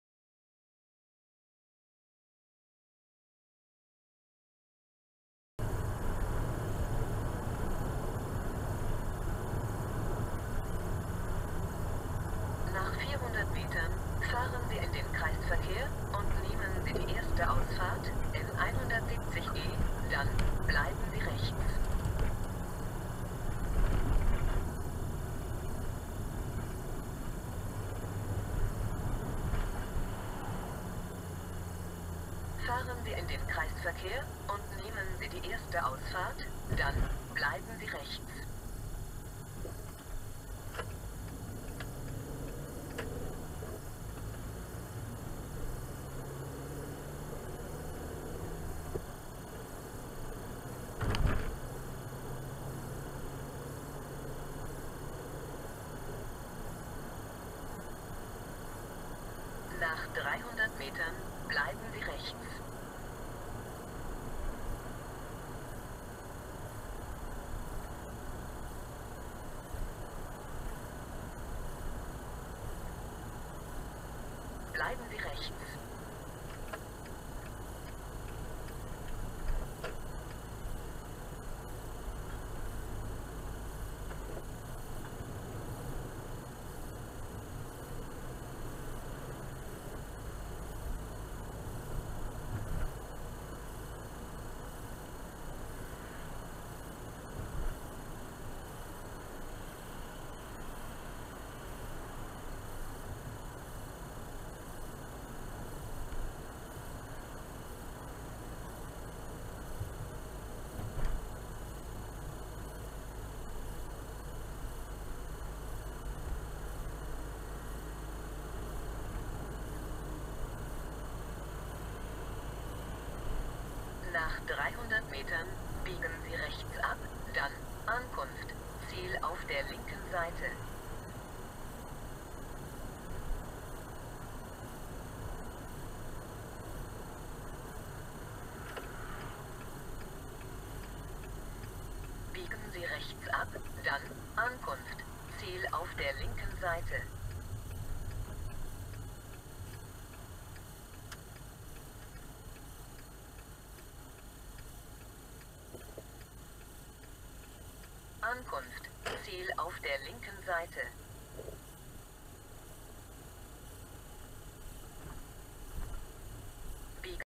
Approach the Naturgy L/CNG station in Carpiquet (F)
We start on D170 south of the N13.
- At the roundabout directly before N13, take the exit to the east "Toutes directions / Carpiquet"
- after 550 m, keep right, don't take the N13
- after one further km ...
the station is on the right side, the vertical LNG tank is visible early.

If you come from N13, you can leave N13 east of the station and approach it from the other side.

The burnt-in timestamp in the video is wrong, it was recorded on August 21st actually, not 22nd.